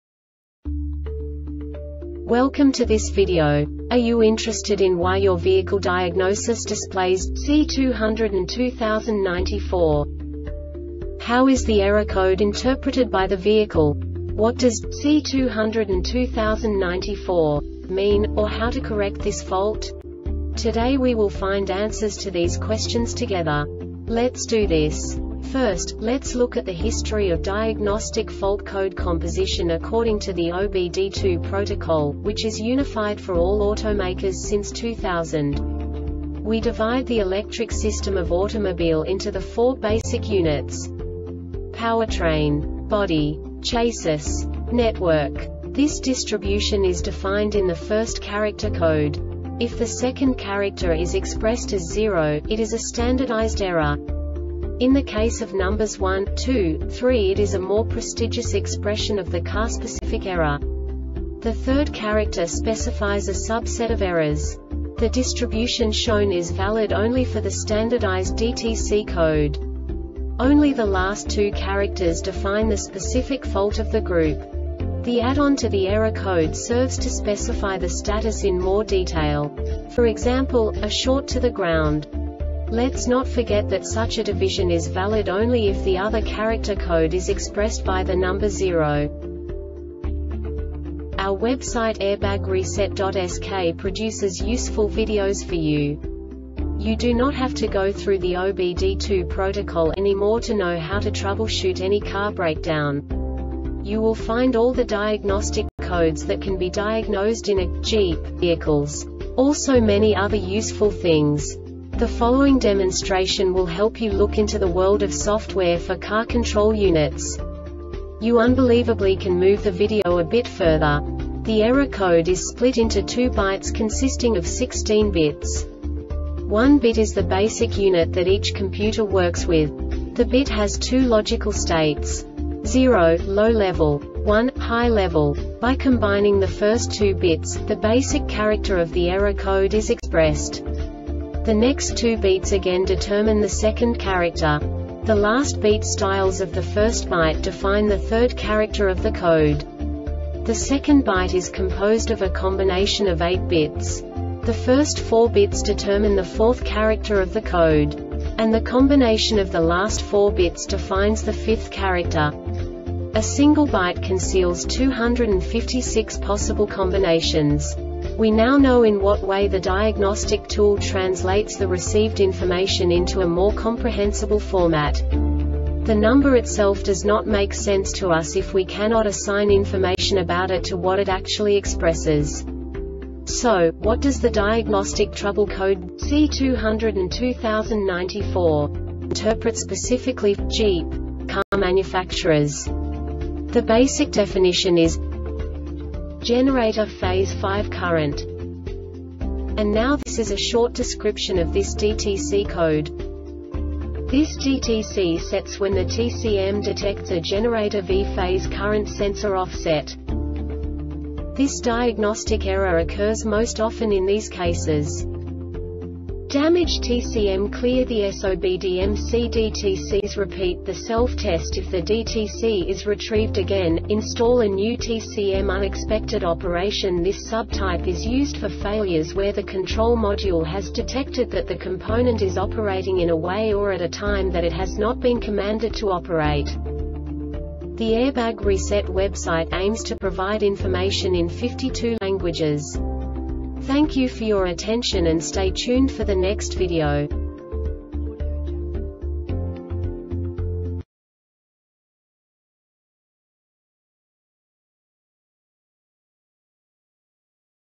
DTC Jeep C2020-94 Short Explanation
Contents:
0:21 Basic DTC analysis according to OBD2 protocol standard.
1:48 Insight into programming
2:58 A diagnostically understandable description of the Diagnostic Trouble Code
3:05 Basic error definition

Make:
Jeep
Code:
C2020 94
Definition:
Generator Phase V Current
Description:
This DTC sets when the TCM detects a generator V phase current sensor offset.
Cause:
Damaged TCM
Diagnosis:
Clear the SOBDMC DTCs. Repeat the self-test. If the DTC is retrieved again, install a new TCM.
Failure Type:
Unexpected Operation
This sub type is used for failures where the control module has detected that the component is operating in a way or at a time that it has not been commanded to operate.